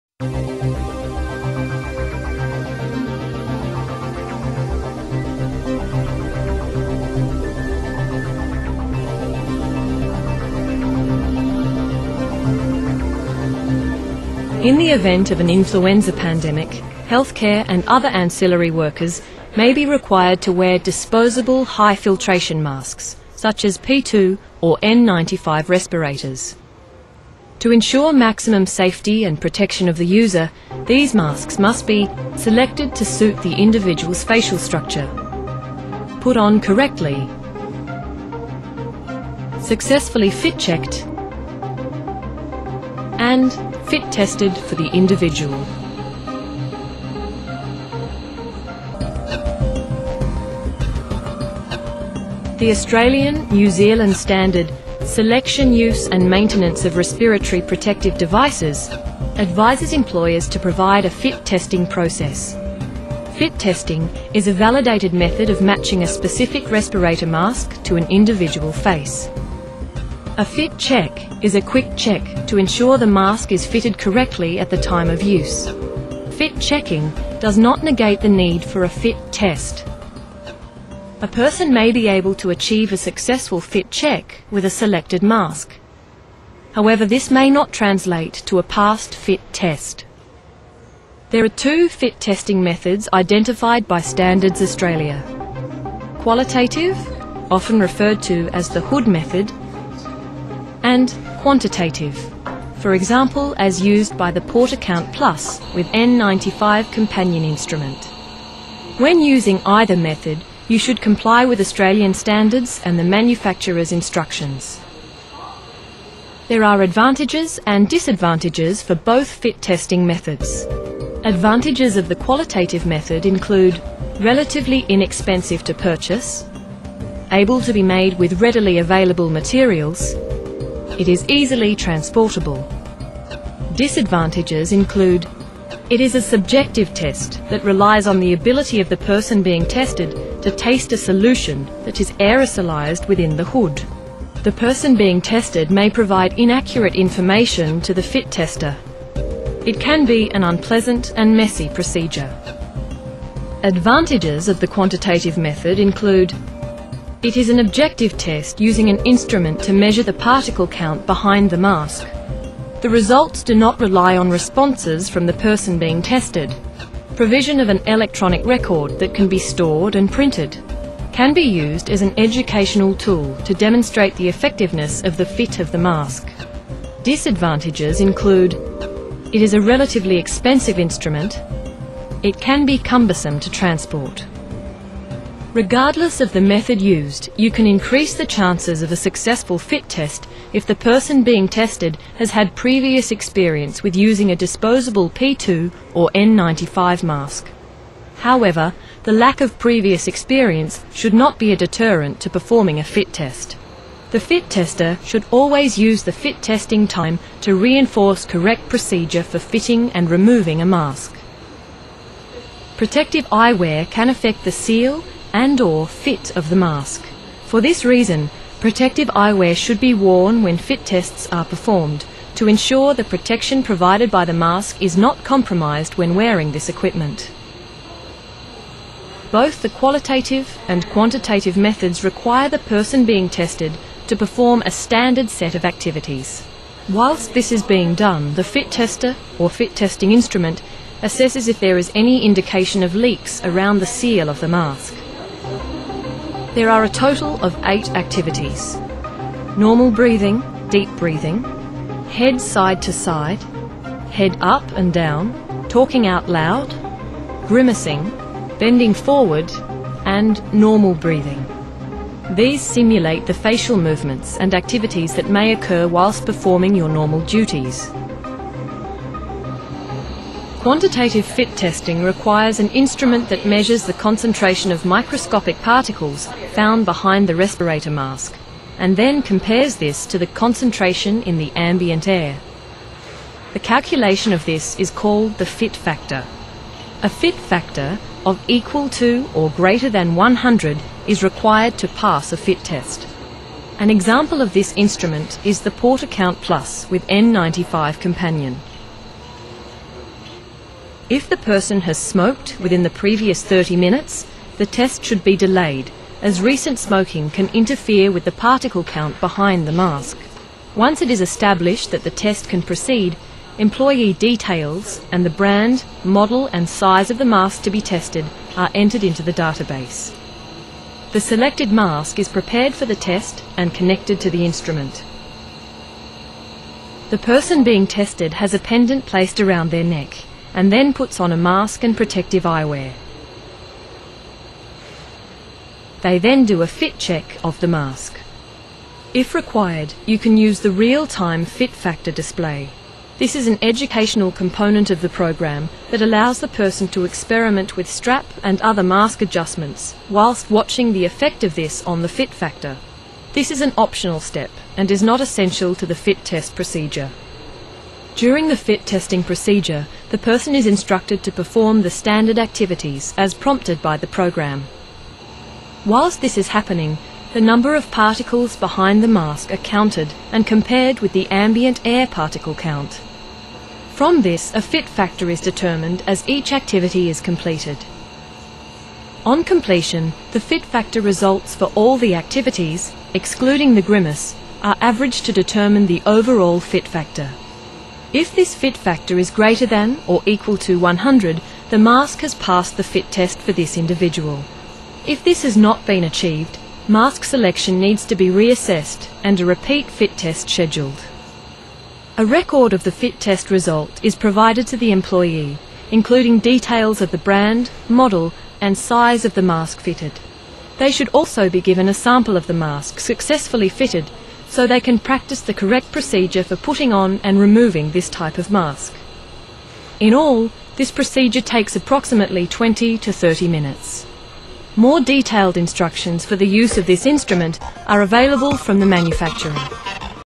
Influenza Respirator Fit Testing Portacount Quantitative Method
Sadly, healthcare workers worldwide are often not getting the protection they need from airborne disease,such as the current H1N1.  In this current swine flu outbreak, CDC began suggesting that surgical masks will do to protect healthcare workers (without the nylon stockings mentioned in this song).  Much evidence regarding airborne transmission and the need for true respirators has accumulated since this video was made.  A recent report by the US Institute of Medicine (IOM) RESPIRATORY PROTECTION FOR HEALTHCARE WORKERS IN THE WORKPLACE AGAINST NOVEL H1N1 INFLUENZA A ) supports that health care workers who interact with patients suspected or confirmed to be infected with novel H1N1 influenza Ashould wear fitted N95 respirators, which filter better than looser medical masks.   However, wearing N95 respirators should be only one element of workers' and health care organizations' infection control strategies.  This video shows the importance of respirator fit testing for healthcare workers who will be exposed to H1N1 (swine) flu.  It also demonstrates the quantitative fit testing protocol, (using the TSI Portacount machine) one of two methods generally approved for employers to use.  Check with your occupational health and safety agency for their specific requirements.  Without passing a fit test, a respirator will not provide the expected protection and the wearer will be at an increased risk of becoming ill from the flu.  We need our healthcare workers to stay healthy to be able to keep taking care of us!  This clip is from the wonderful Australian Government Department of Health and Agings DVD "Pandemic Infuenza Preparedness, Safe Use of Personal Protective Equipment (PPE)".  Downloadable clips from this DVD are available